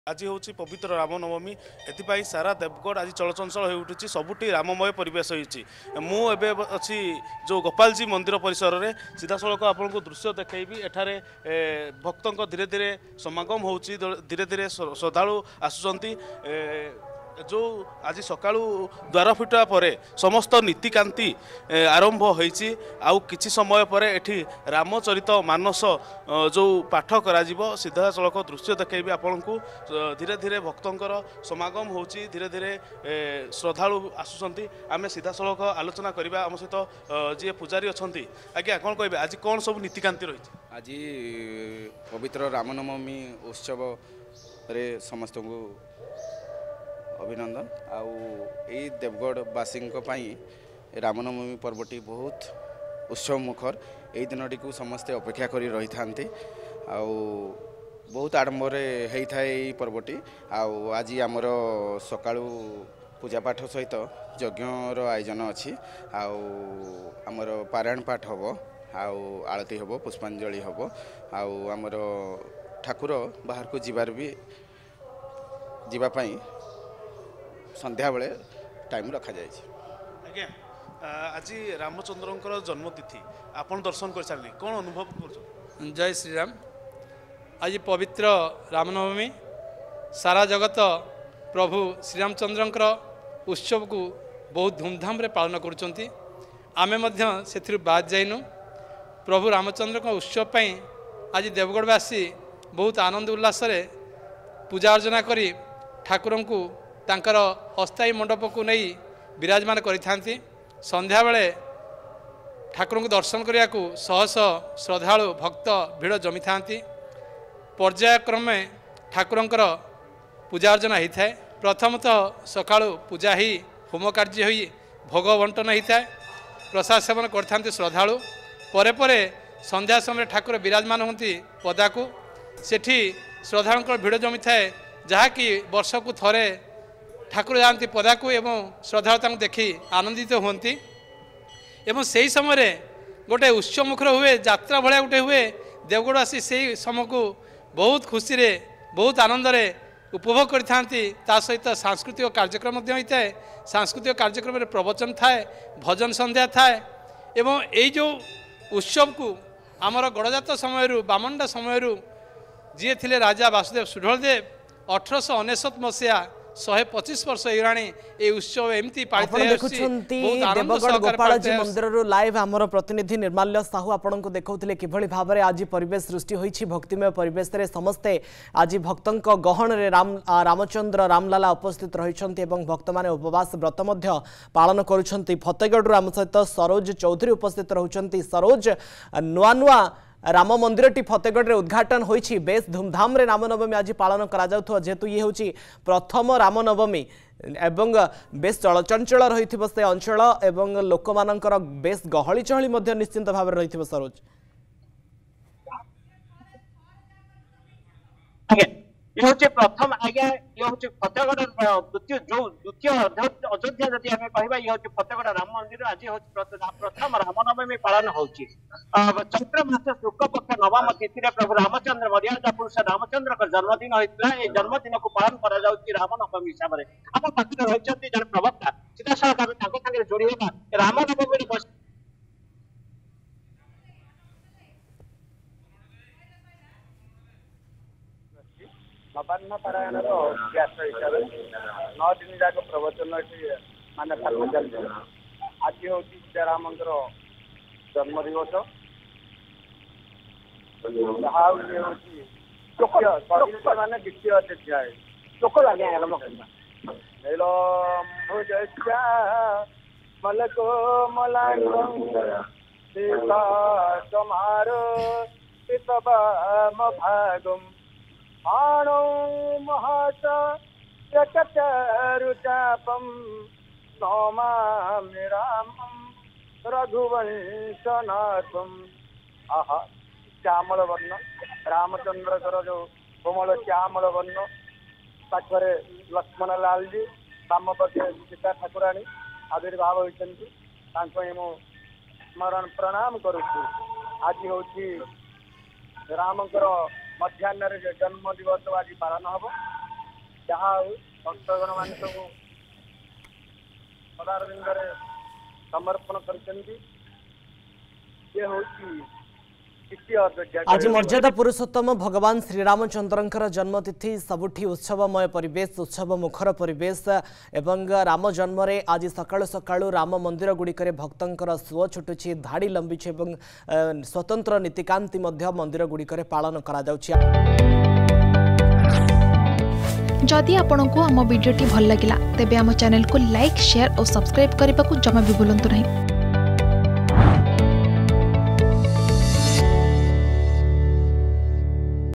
ପ୍ରଭୁ ରାମଚନ୍ଦ୍ରଙ୍କ ଉତ୍ସବକୁ ବହୁତ ଆନନ୍ଦ ଉଲ୍ଲାସରେ ପାଳୁଛନ୍ତି ଦେବଗଡବାସୀ || Ram Navami 2024
ପ୍ରଭୁ ରାମଚନ୍ଦ୍ରଙ୍କ ଉତ୍ସବକୁ ବହୁତ ଆନନ୍ଦ ଉଲ୍ଲାସରେ ପାଳୁଛନ୍ତି ଦେବଗଡବାସୀ ।